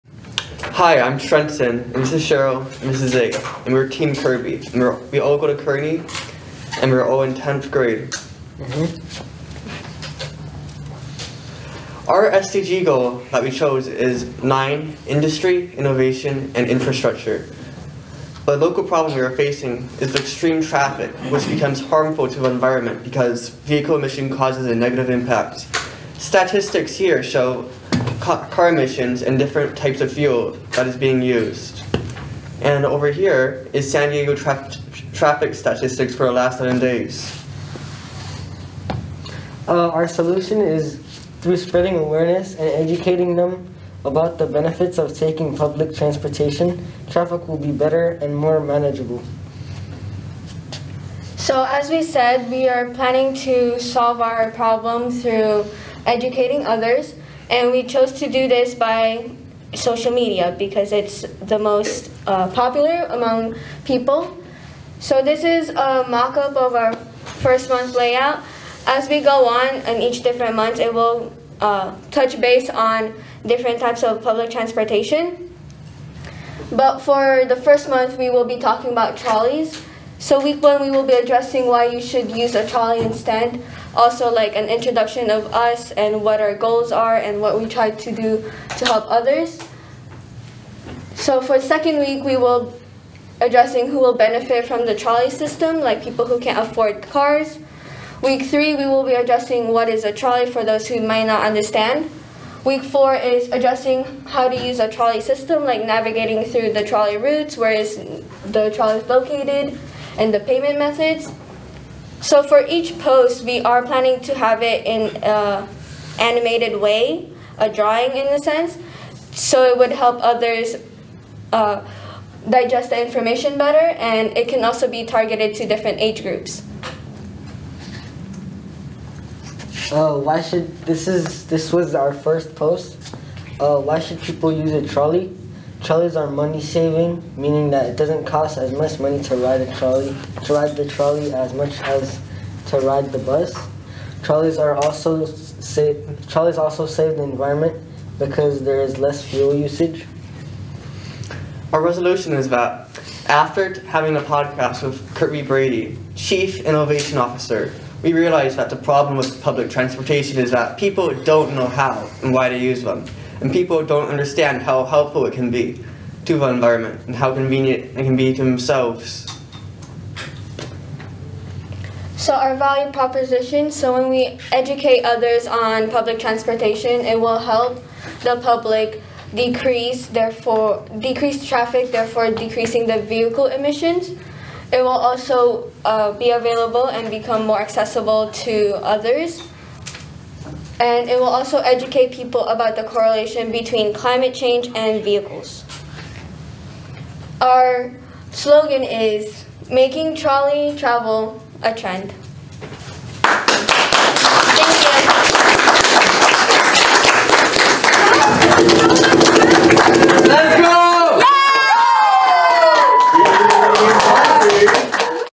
Team Kirby Final Pitch - ImPactful Teen Innovation Sandbox
Educate people on public transportation system to decrease traffic.